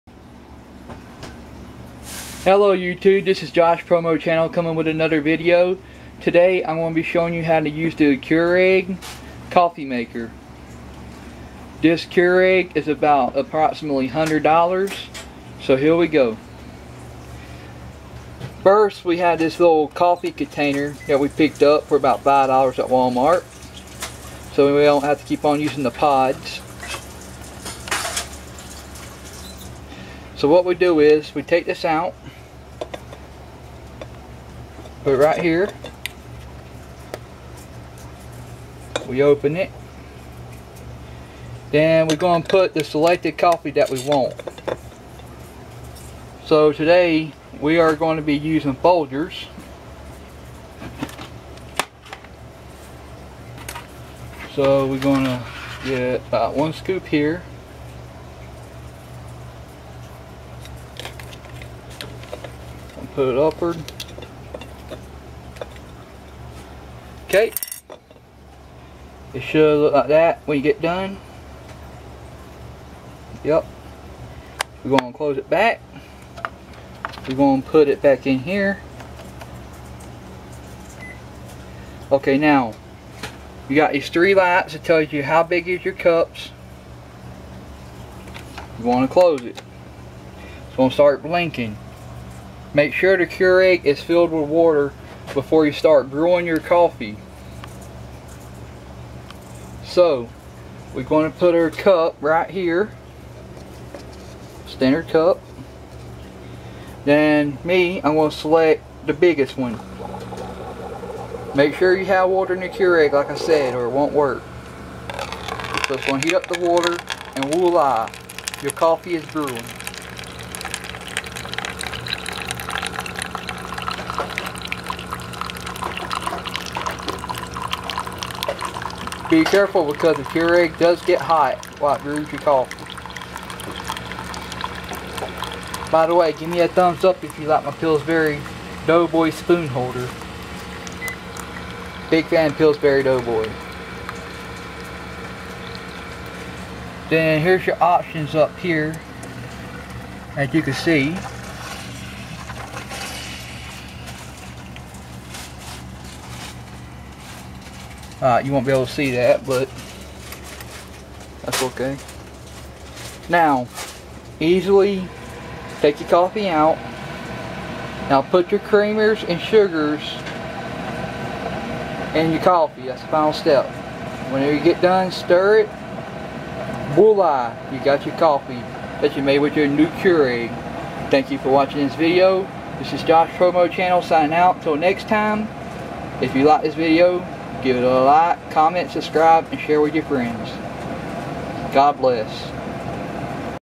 How to use a Keurig® K-Classic® Coffee Maker for beginners
In this video, I will be explaining how to use a Keurig® K-Classic® Coffee Maker.